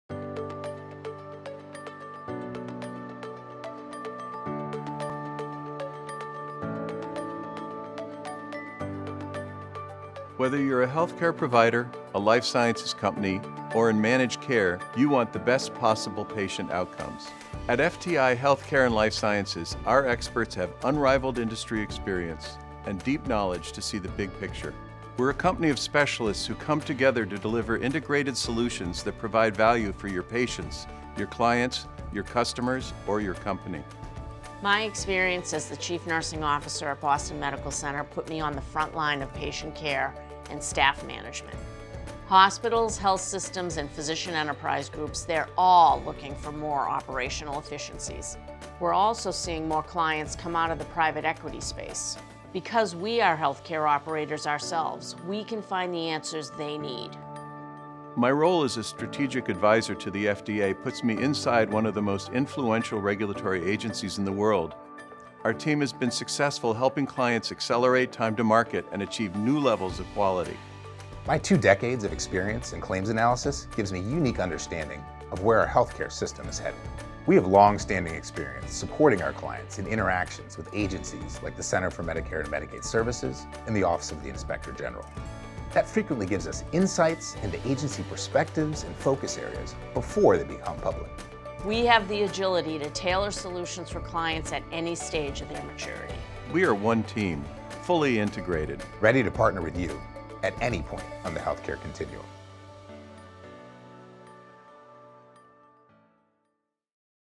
FTI Consulting Healthcare & Life Sciences
Whether you’re a healthcare provider, a life sciences company or in managed care, you want the best possible outcomes for your patients. Meet a few of FTI Consulting’s Life Sciences & Healthcare experts, who have years of hands-on industry experience and are dedicated to supporting clients across the value chain.